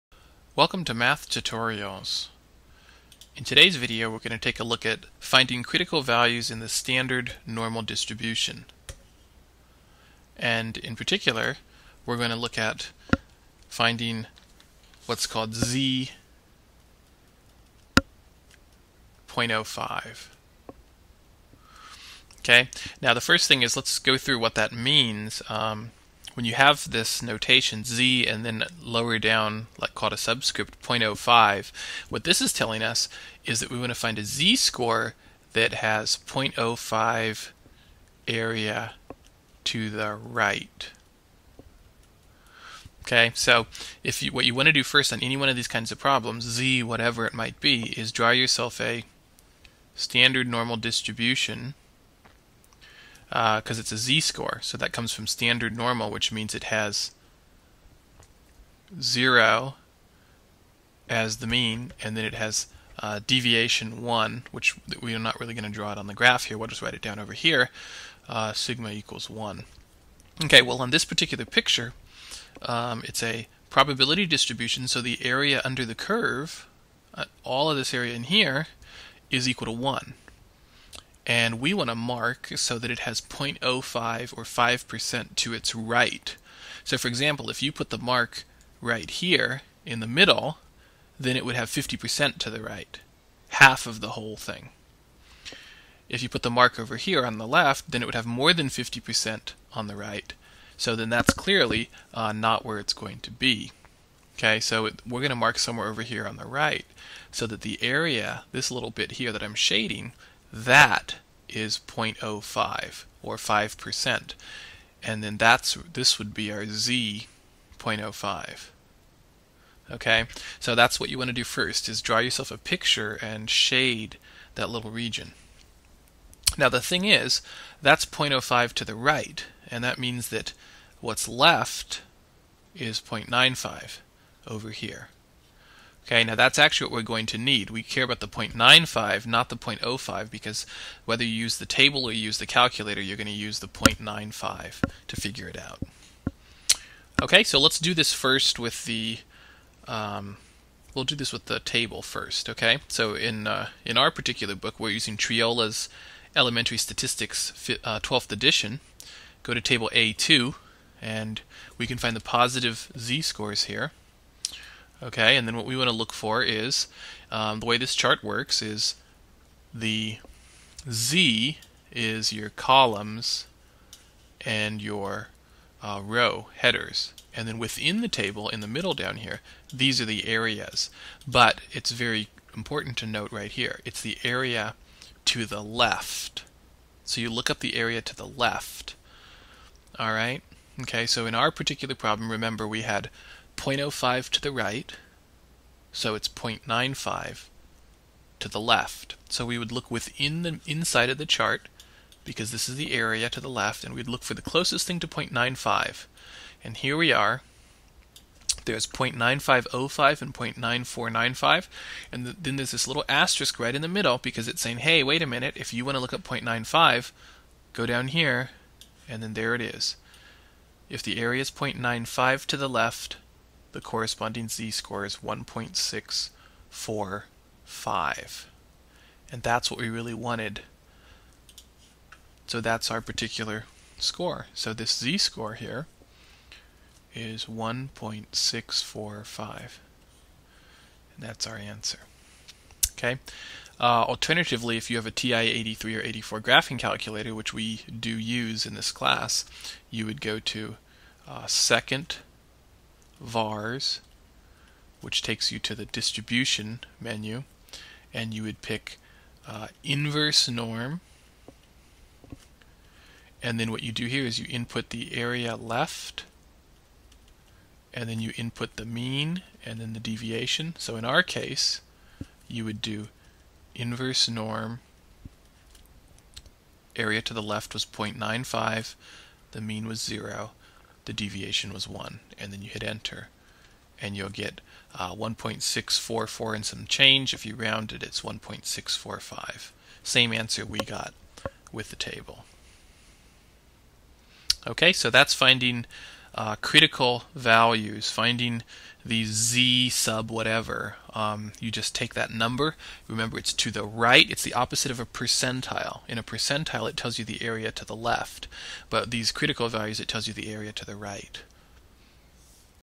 Find Critical Value in Standard Normal Z Distribution
How to find a critical value z.05 in the Standard Normal N(0,1) Distribution, using either the Table or your TI-83/84 Graphing Calculator.